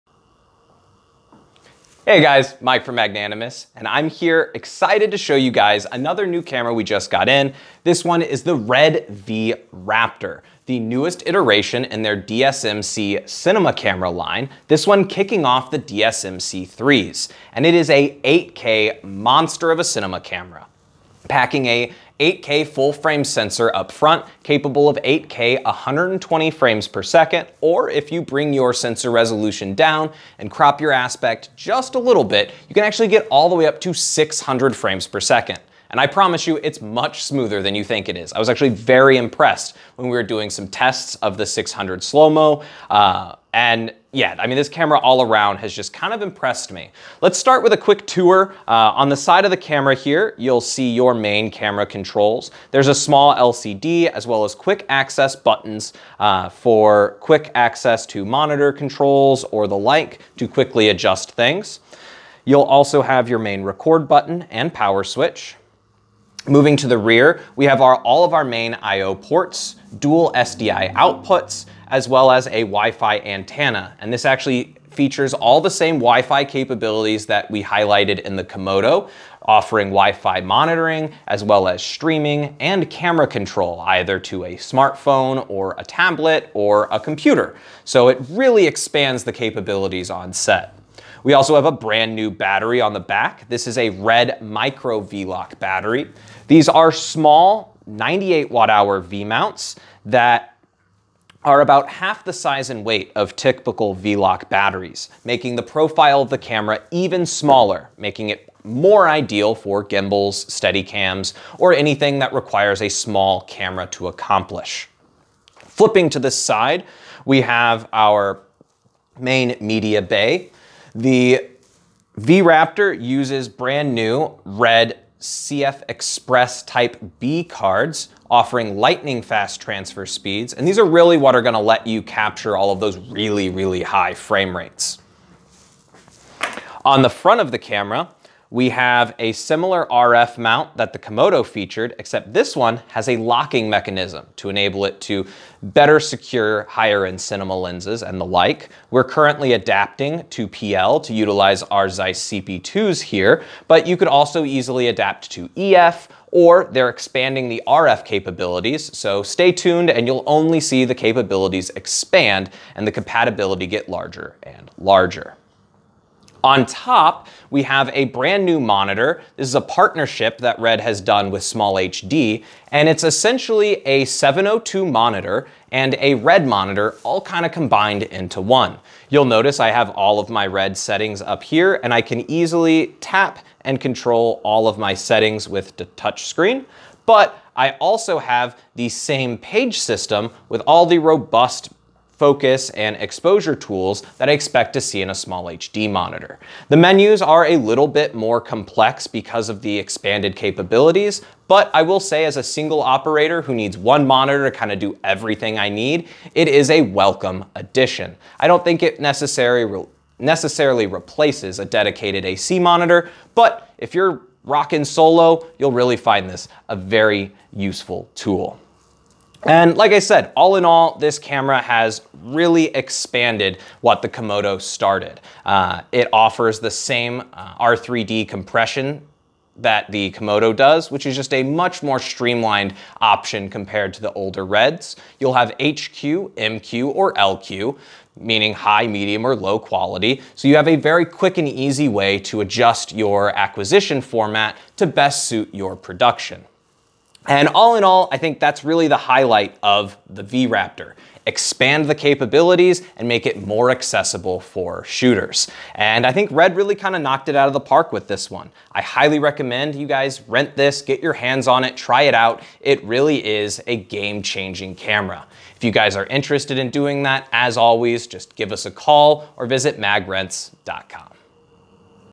RED V-RAPTOR 8K VV Body Kit is in Stock! - MagRents.com
RED V-RAPTOR 8K VV Body Kit is now in stock! Mike gives you a quick walk through.

Compact, fast, and full-frame—the V-RAPTOR 8K VV + 6K S35 Dual-Format Camera is RED DIGITAL CINEMA's impressive introduction to their new DSMC3 platform. Sized only slightly larger than the KOMODO but boasting a new sensor, the V-RAPTOR offers multi-format capture in 8K VV, 6K Super35, 4K, 3K Super16, and anamorphic options. This versatility enables you to adapt your format from project to project to suit your choice of a variety of pro cine or still photo lenses.